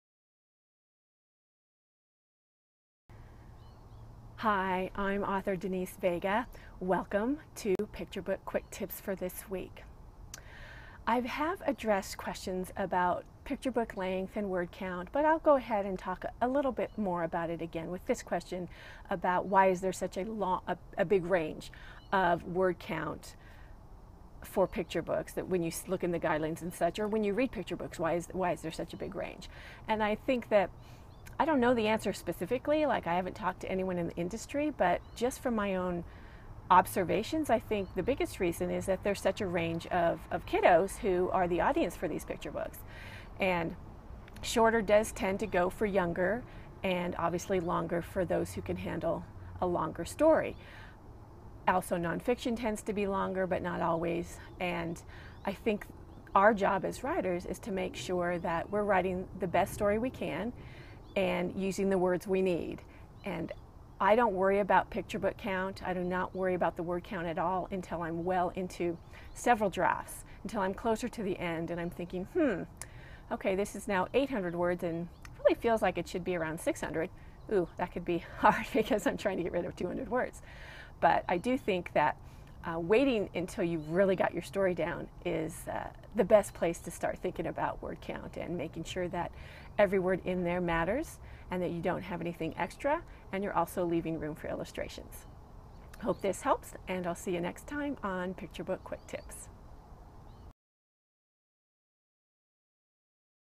Why is there such a large range of word counts for picture books?
If you'd like to learn with me, check out classes, critiques, and consultations